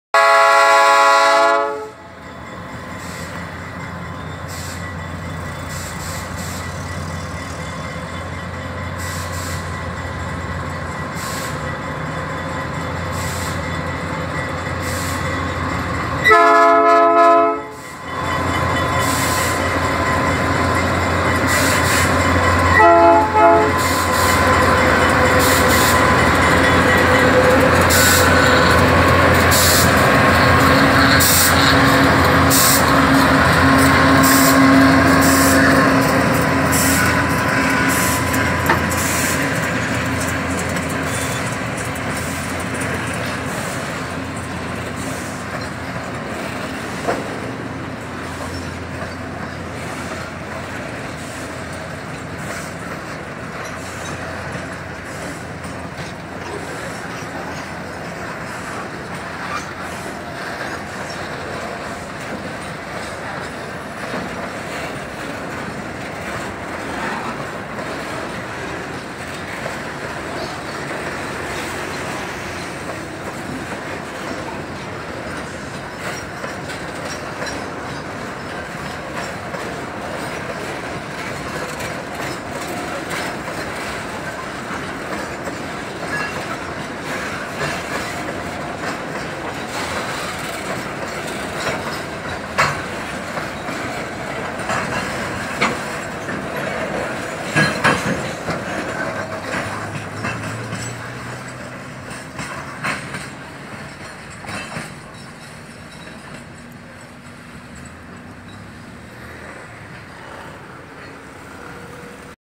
Trem prefixo Y490 cimenteiro passando por Curvelo-MG !
Trem prefixo Y490 gravado junto ao meu amigo Matheus com locomotivas U20C convencionais da VL! e comum ( 2511 líder ), ( 2514 auxiliar ) e ( 2505 rebocada/desligada ) com 20 vagões TPD carregados de cimento. Trem com destino há Bahia vindo de Vespasiano-MG.
Horário: 07:49
CANAIS PARCEIROS: